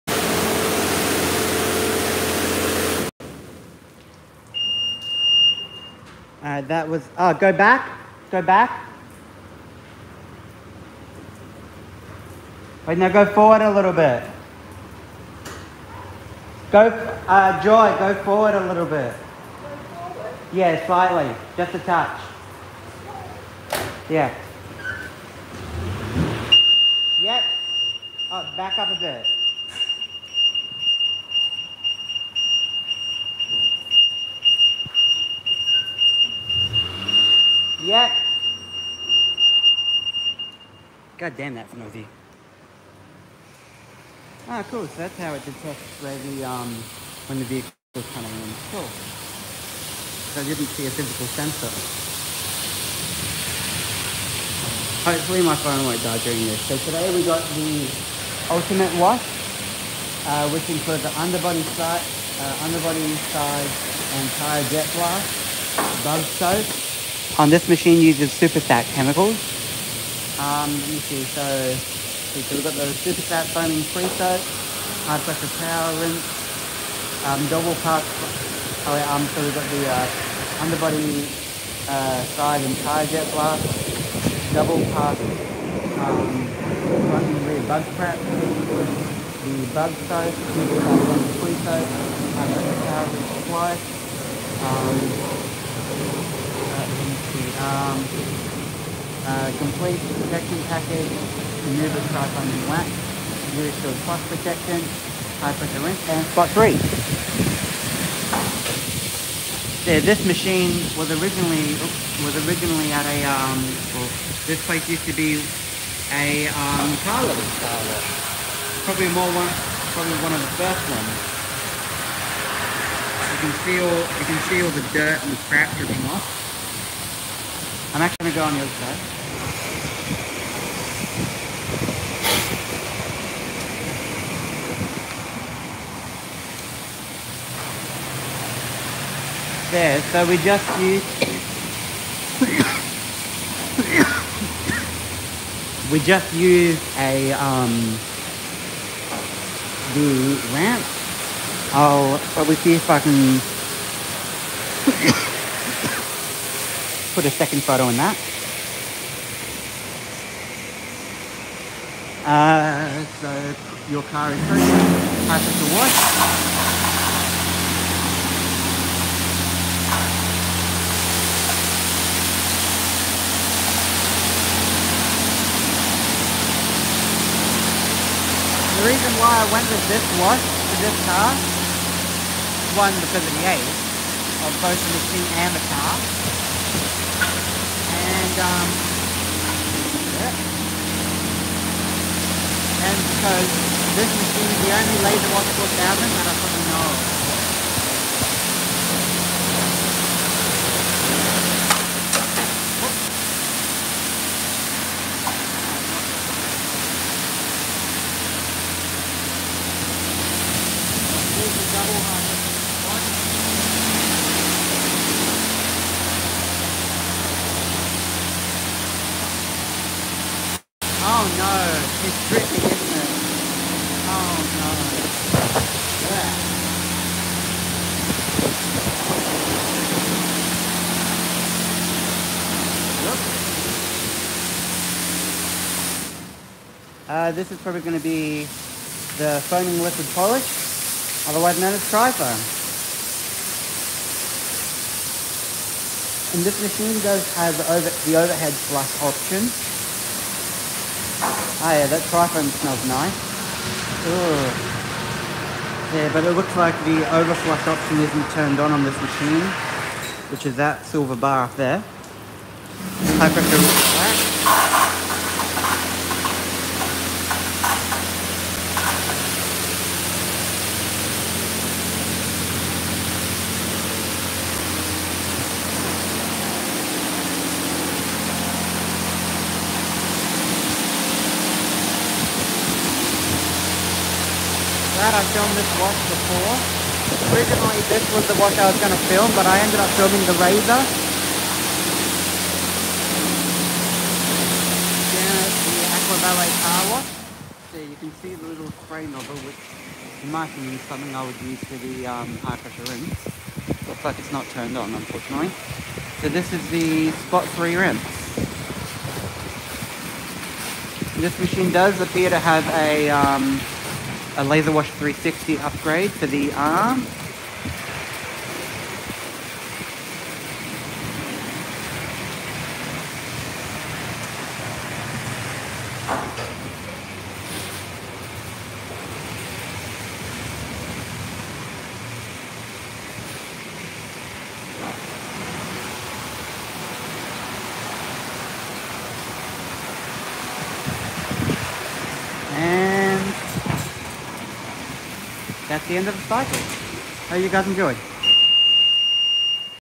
PDQ Laserwash 4000
This machine appears to be original to the building and has a laserwash 360 arm cover, it appears the overhead flush isn’t working sadly and the car did leak  but 8/10 for wash quality and they had a diy underbody wash ramp where I put wheel cleaner and presoak on before the wash, and they had a powerful vacuum